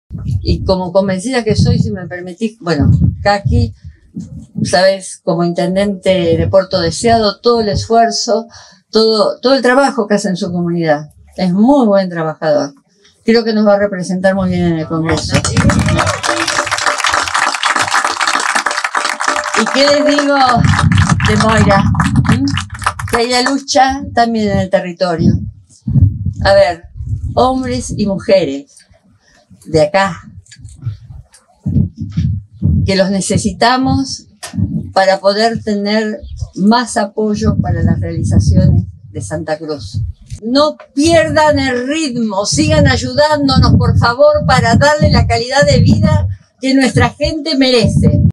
27 de septiembre de 2021
Alicia Kirchner inaugura red de gas junto a candidatos del Frente de Todos